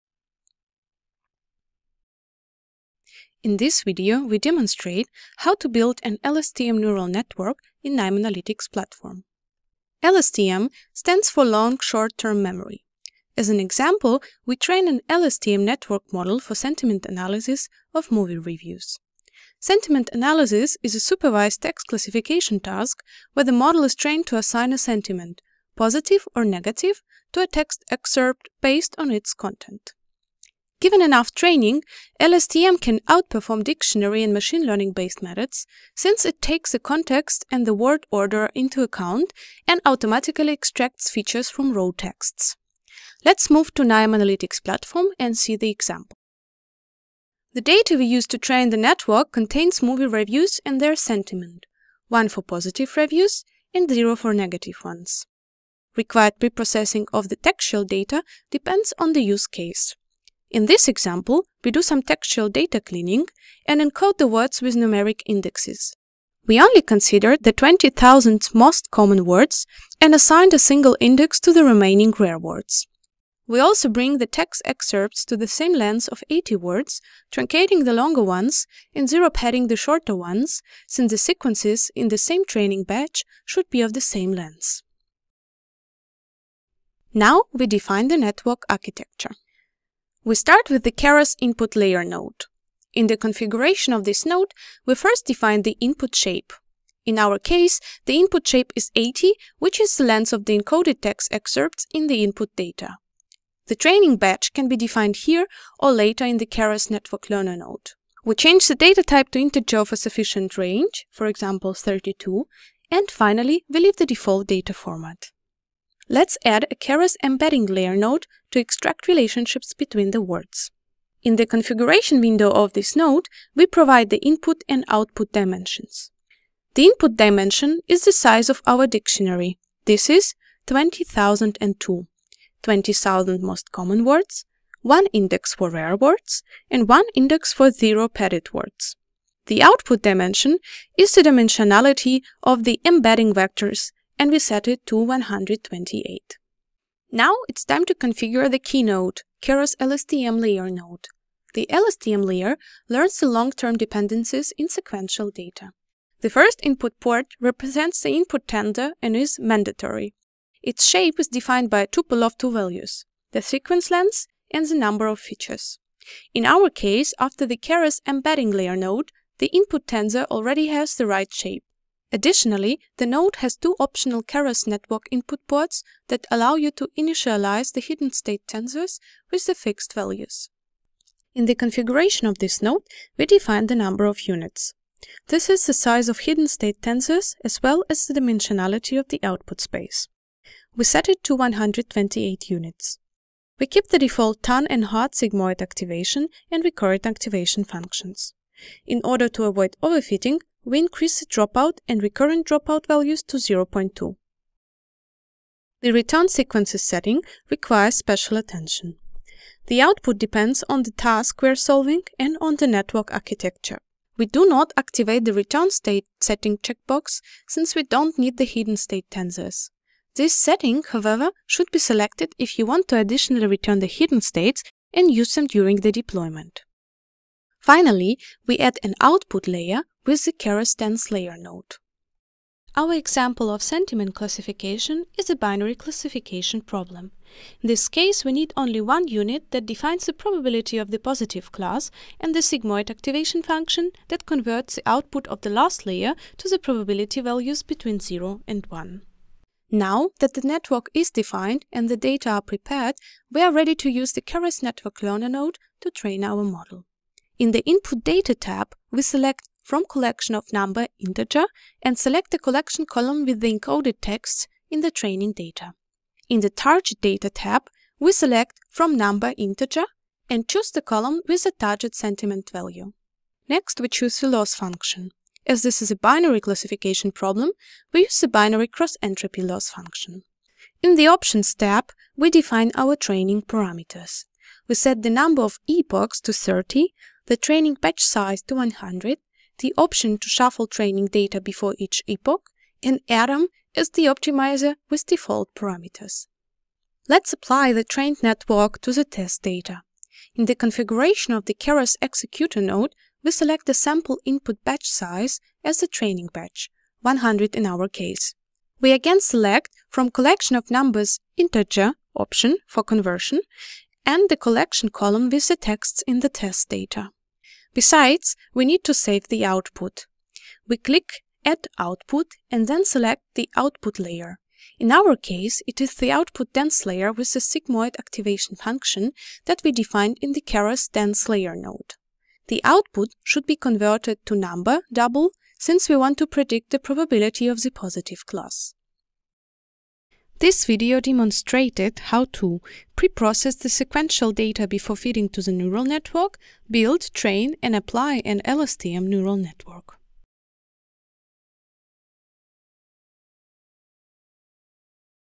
How to train an LSTM model in KNIME
The video shows how to build an LSTM neural network based on sequential data and apply it for text classification.

The workflow, shown in the video, can be accessed on the KNIME Community Hub via the